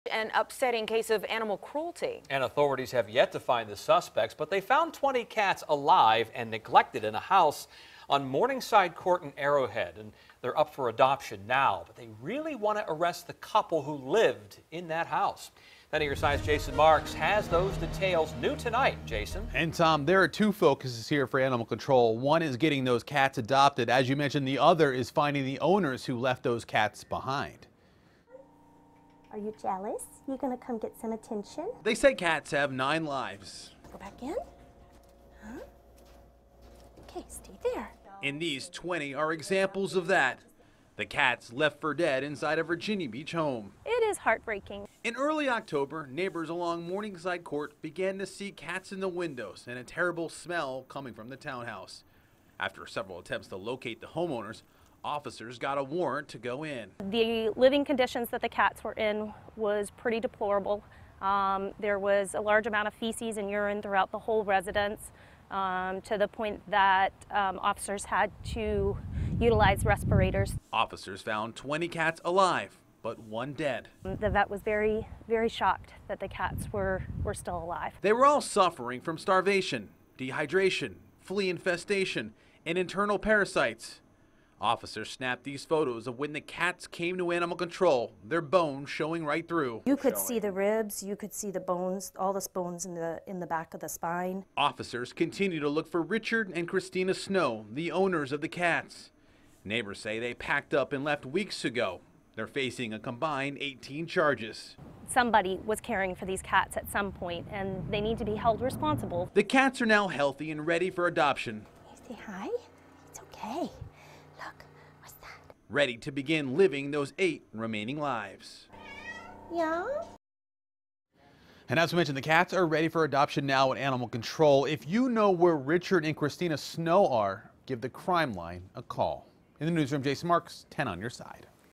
21 cats, one dead, removed from Va. Beach home, police say
Jason Marks reports.